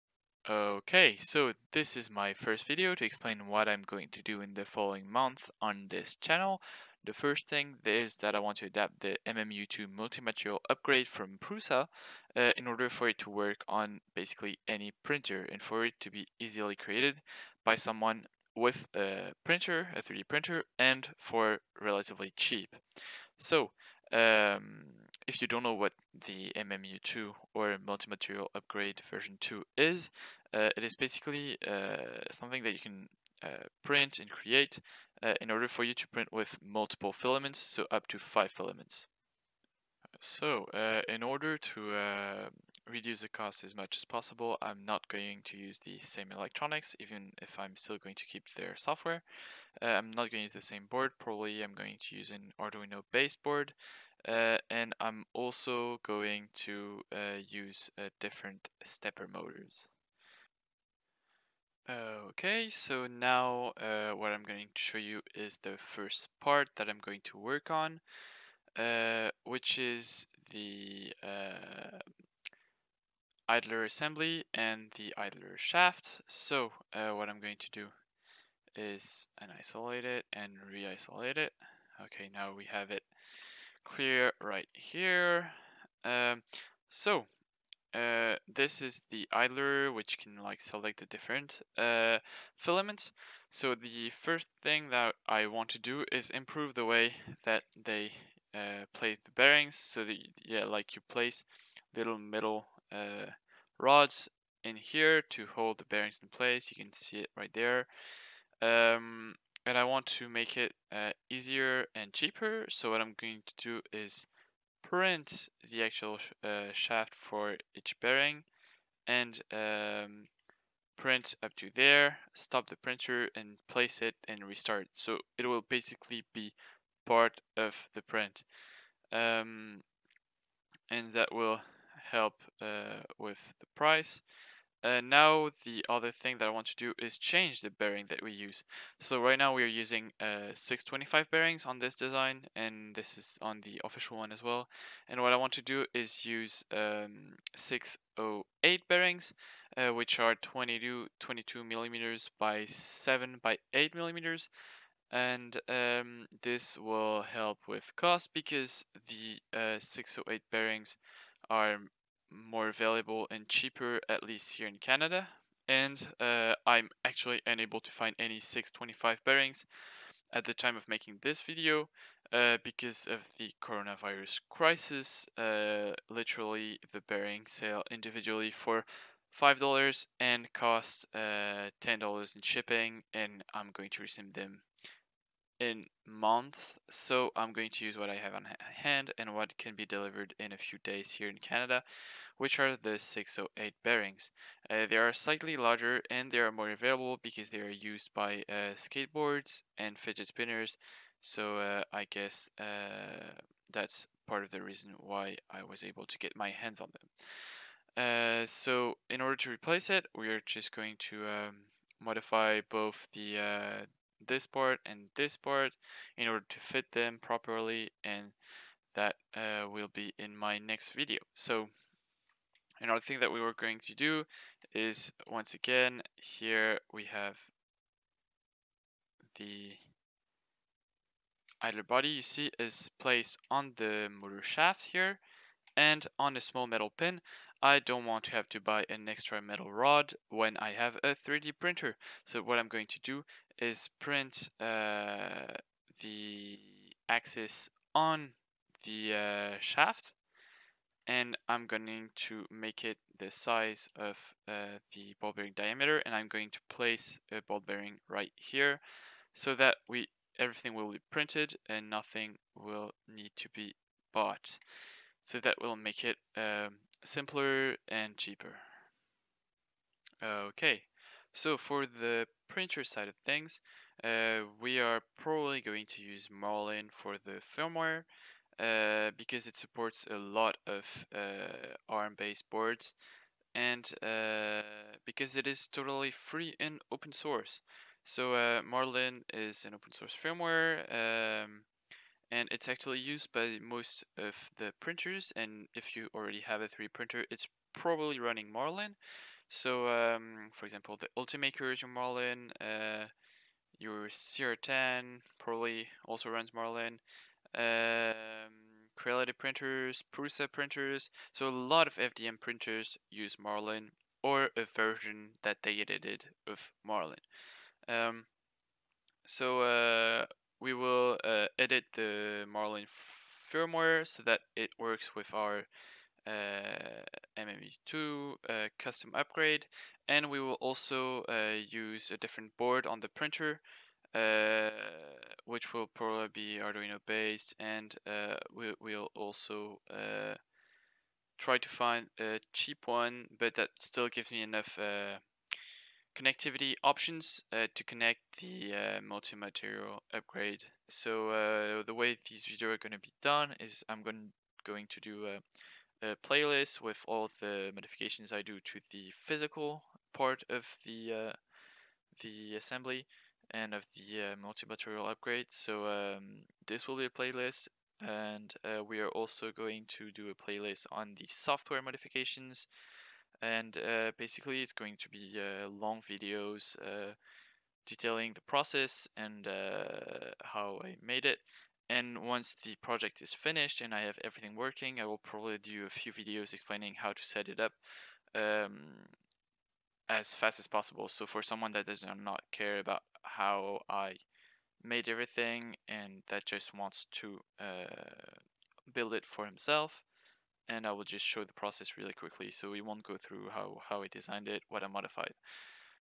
MMU2 upgrade on any printer Part 1: introduction
This an introduction to one of my new projects, redesigning and modifying the MMU2 upgrade from prusa and make it cheaper and compatible with other printers.

Support any support goes directly back into this project: parts, filament, video-related gear, ...